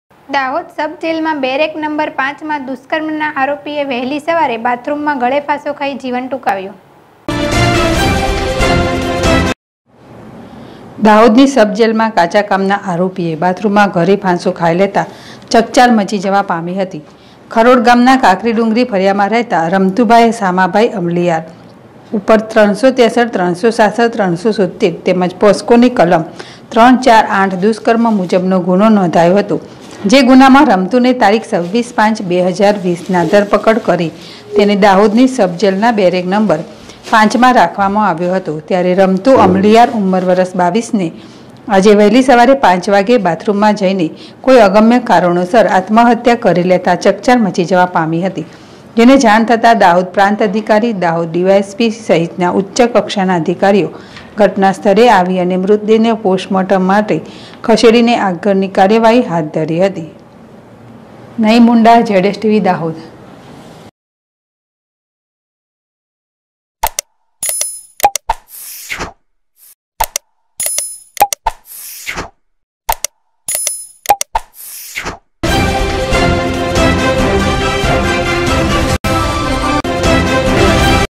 દાહોદ સબજેલ મા બેરેક નમ્બર ૫ મા દુષકર્મ ના આરોપી એ વહેલી સવારે બાથરૂમ મા ગળે ફાંસો
દાહોદ સબજેલ મા બેરેક નમ્બર ૫ મા દુષકર્મ ના આરોપી એ વહેલી સવારે બાથરૂમ મા ગળે ફાંસો ખાઇ જીવન ટુંકાવ્યુ